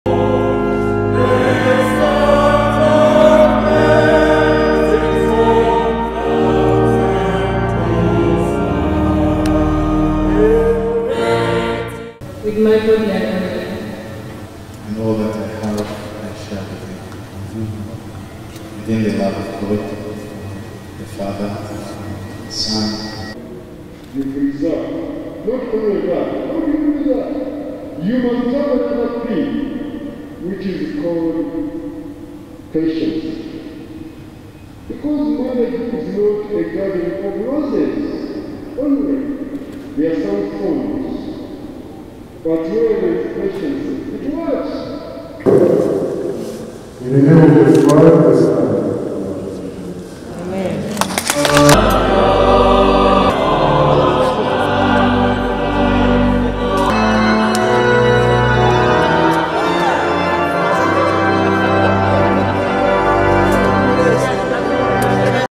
NTV’S John Cliff Wamala weds Saphira
One of our own, John Cliff Wamala has exchanged marriage vows with his wife Saphira at St Paul's Cathedral Namirembe.

The new couple were wed by the Archbishop of the Church of Uganda, Dr. Stephen Kaziimba Mugalu who encouraged them to Love each other.